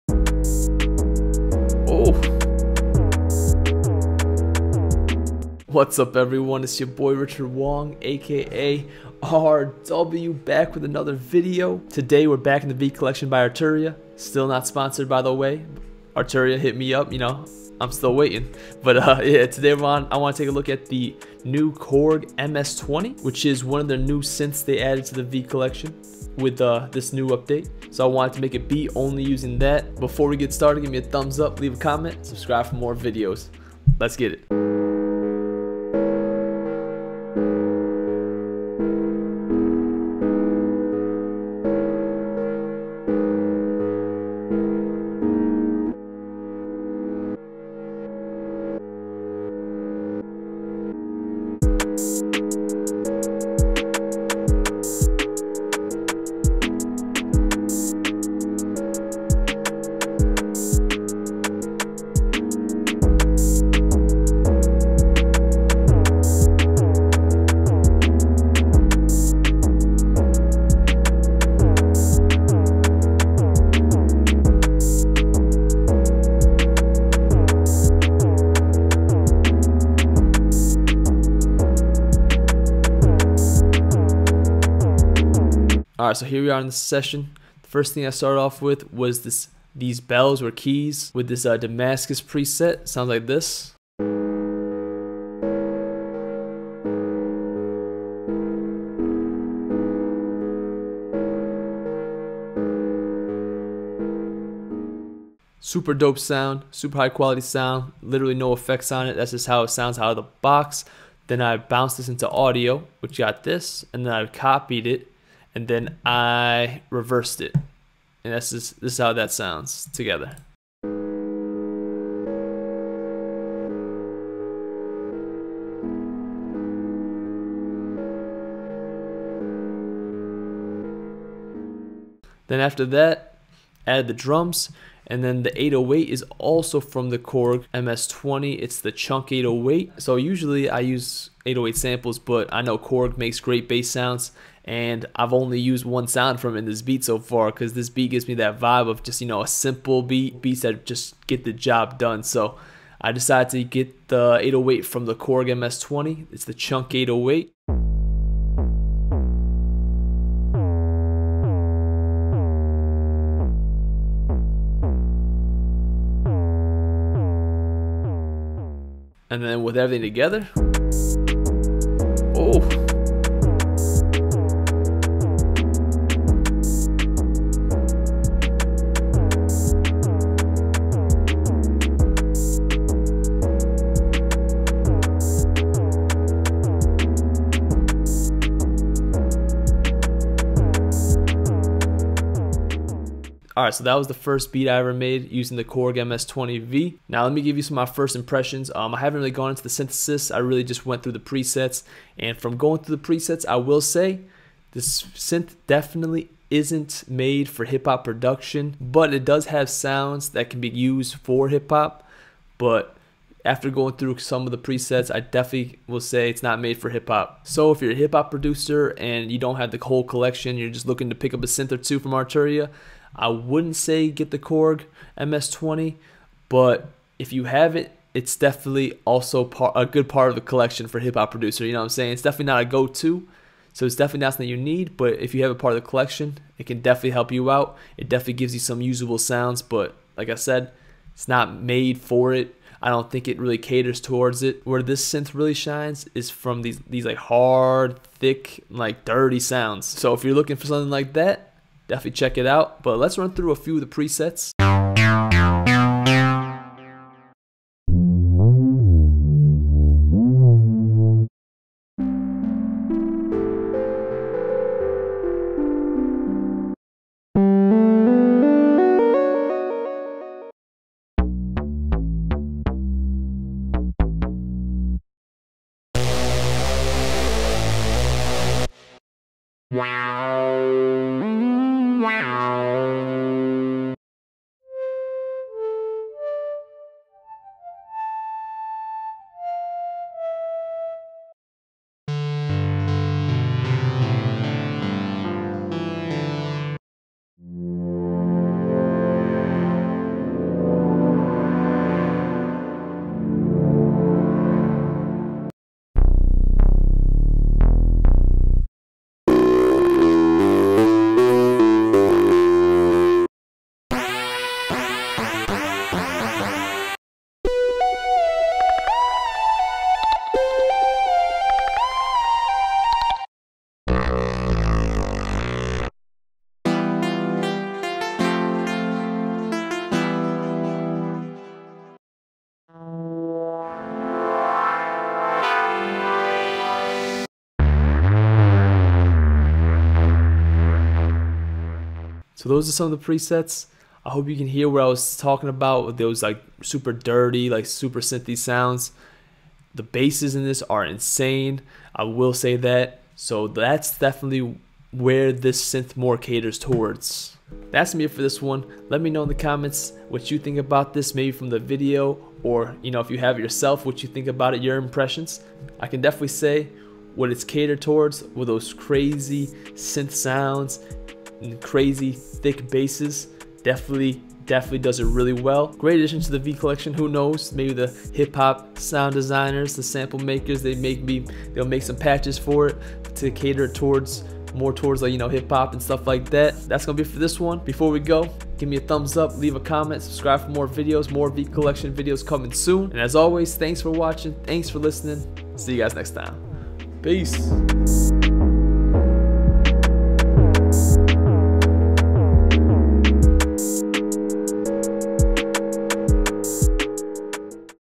Arturia Korg MS-20 For Beat Making? Making a Beat From Scratch [2022]
In this video I make a beat using the new Arturia Korg MS-20 and give my thoughts on the plugin

0:00 "Intro"
0:34 "Making the Beat"
1:25 "Beat Breakdown"
3:13 "For Beat Making?"
4:14 "Presets"
5:57 "Outro"